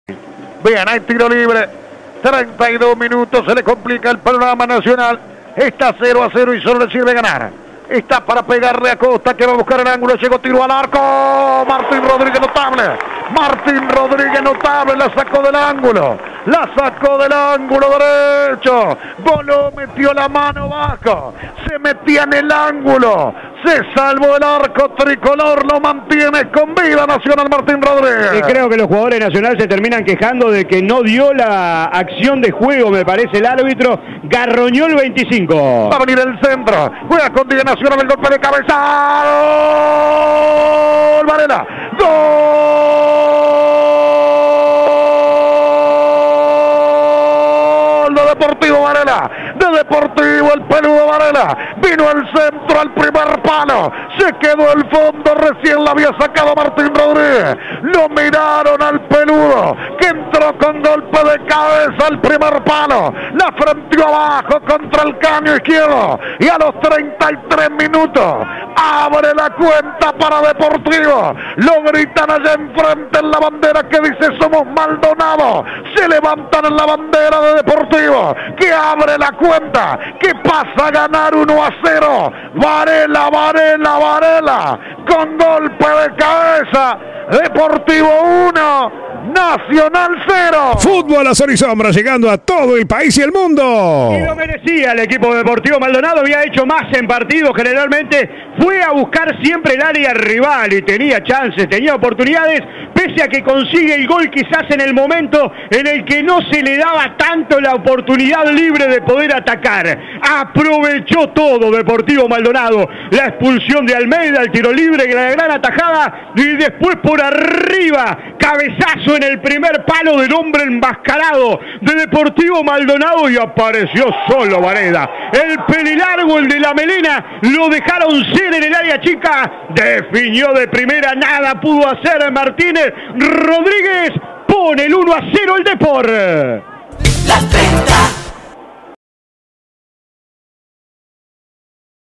Deportivo Maldonado 1 Nacional 0 - Clausura 2021 (por Eduardo Gutiérrez)
El gol de Varela, narrado por Eduardo Gutiérrez y comentado por Alfonso Irrazábal para Radio Nacional.